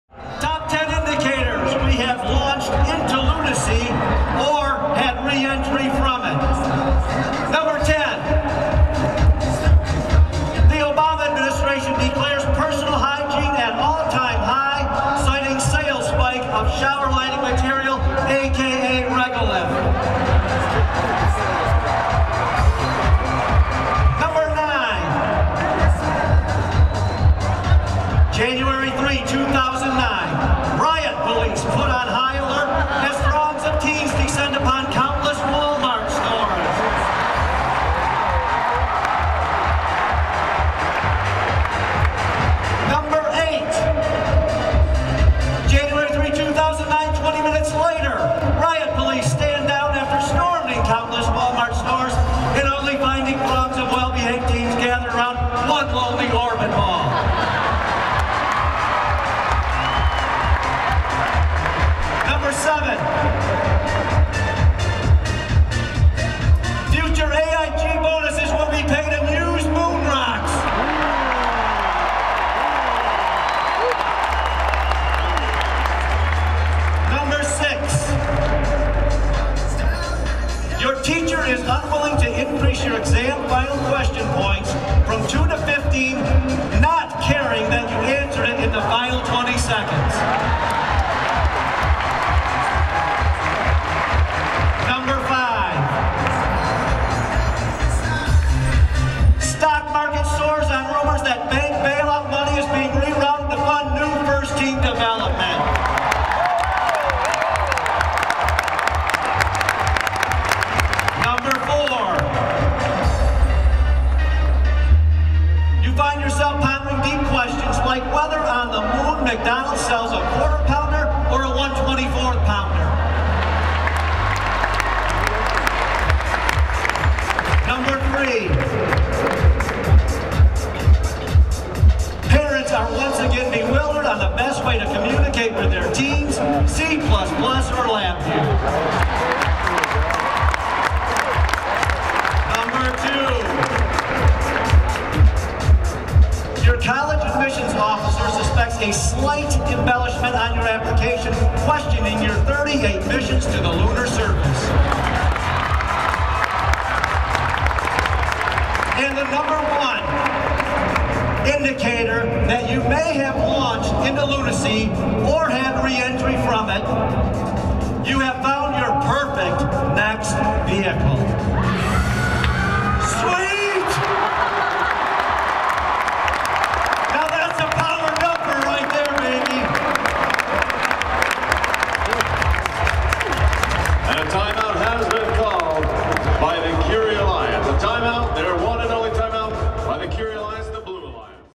FIRST Robotics 2009 - Top 10 Indicators We Have Launched into Lunacy or had Re-entry from It.
10) Obama administration declares personal hygiene at all time high citing sales spike of shower lining material aka Regolith.

9) January 3, 2009: Riot police put on high alert as throngs of teens descend upon countless Walmart stores.

8) January 3, 2009: 20 minutes later riot police stand down after storming countless Walmart stores and only finding throngs of well behaved teens gathered around one lonely Orbit ball.

7) Future AIG bonuses will be paid in used moon rocks.

6) Your teacher is unwilling to increase your exam final question points from 2 to 15 not caring that you answered it in the final 20 seconds.

5) Stock market soars on rumors that bank bailout money is being re-routed to fund new FIRST team development.

4) You find yourself pondering deep questions like whether on the moon McDonalds sells a 1/4 pounder or a 1/24 pounder?

3) Parents are once again bewildered on the best way to communicate with their teens. C++ or LabVIEW?

2) Your college admissions officer suspects a slight embellishment on your application questioning your 38 missions to the lunar surface.

1) You have found your perfect next vehicle. (see video)